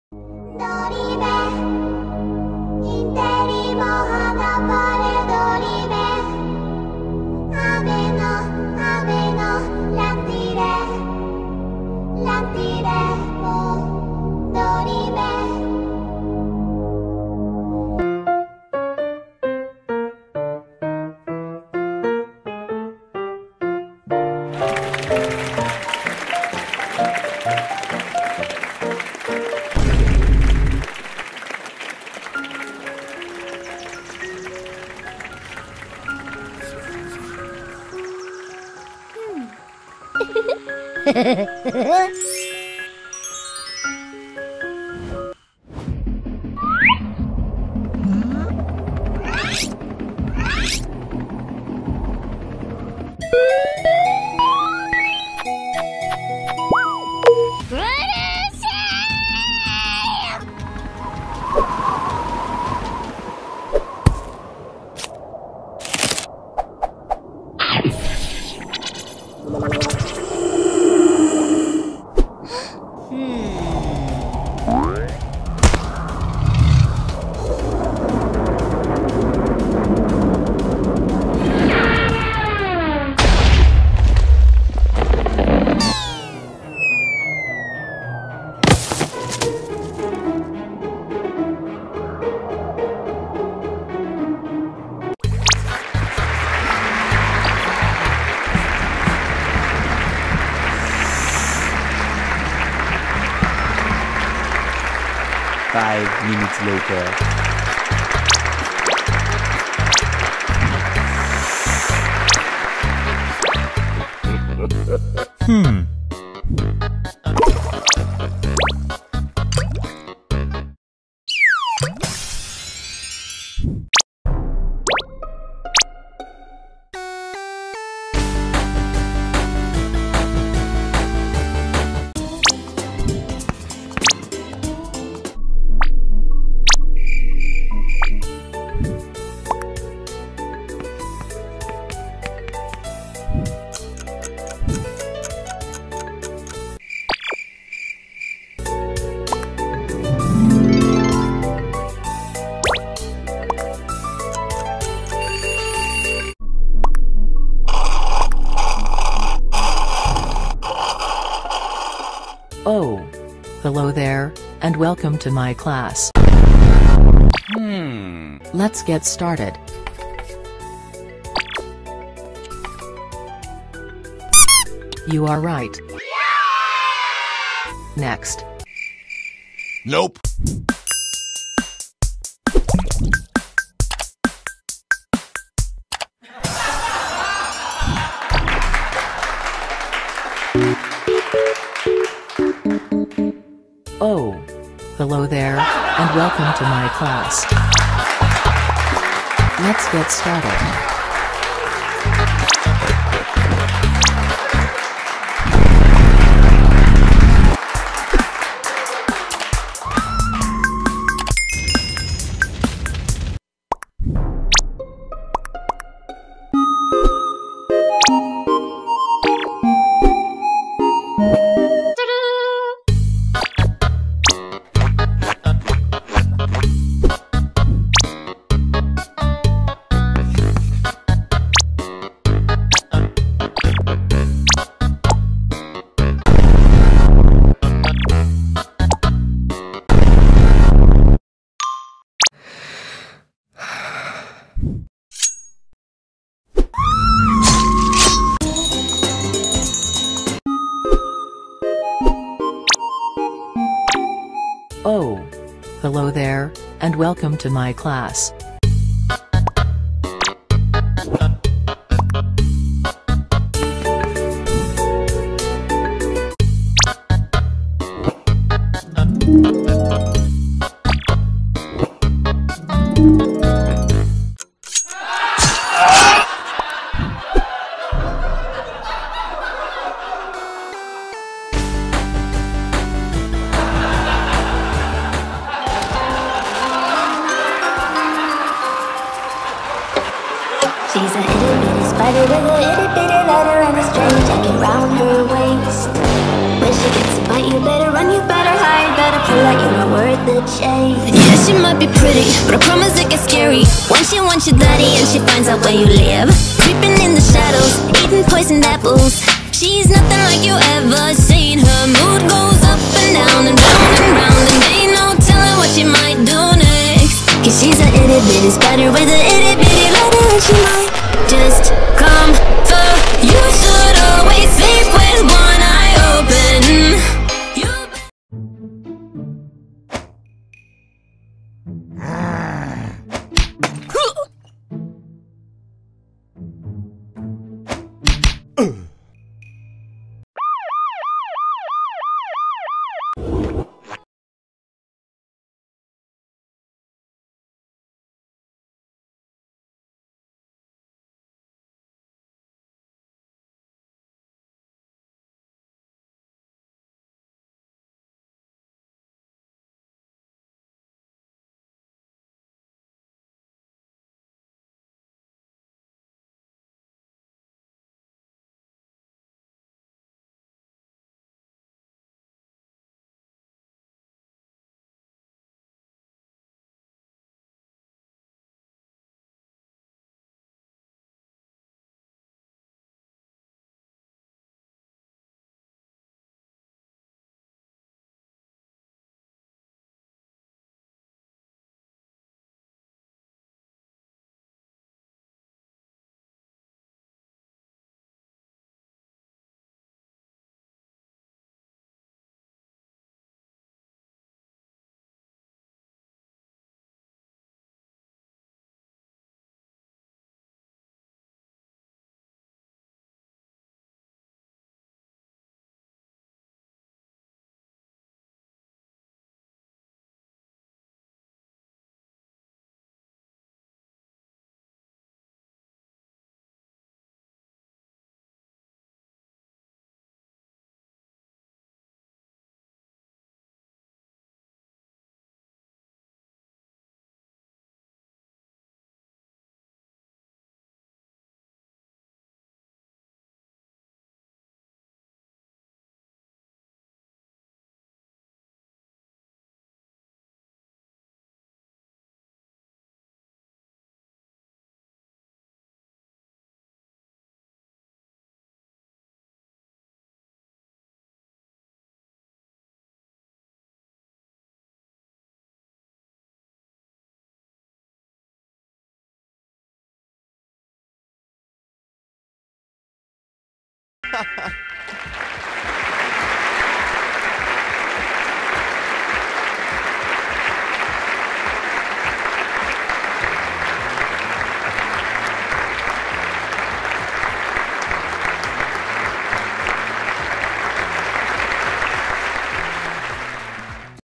Garten of Banban 360° Nab Nab & Chef Pigster react to Garten of Banban meme 2 360° - CINEMA HALL
Garten of Banban react to Garten of banban meme 360° - CINEMA HALL | VR/360° Experience